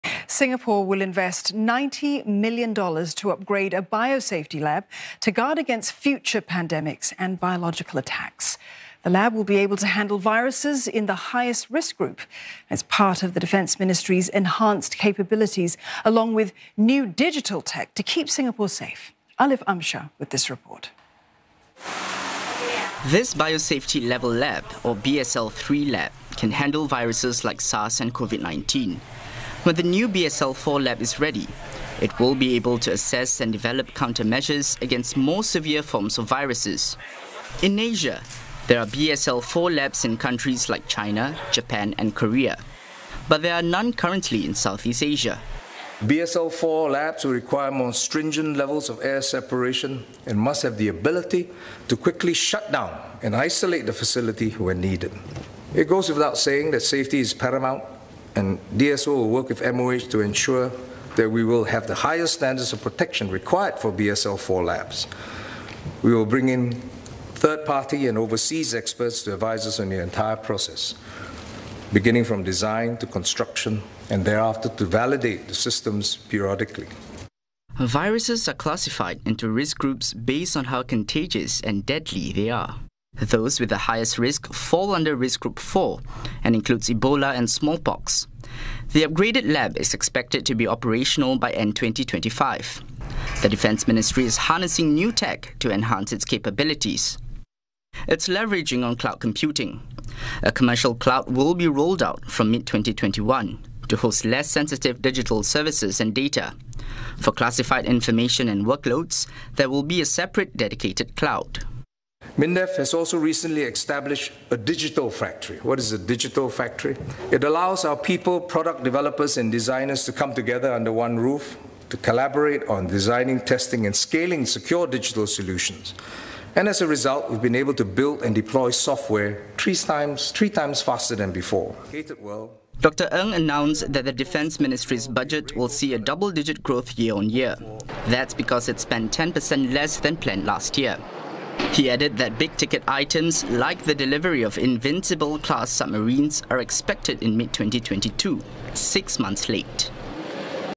Singapore to invest $90m to upgrade biosafety lab to deal with more severe biological threats
Singapore will invest $90 million to upgrade a biosafety lab to guard against future pandemics and biological attacks. The lab will be able to handle viruses in the highest risk group. It's part of the Defence Ministry's enhanced capabilities, along with new digital tech, to keep Singapore safe. When the new BSL-4 lab is ready, it will be able to assess and develop countermeasures against more severe forms of viruses. In Asia, there are BSL-4 labs in countries like China, Japan and Korea. But there are none currently in Southeast Asia. The lab is expected to be operational by end-2025.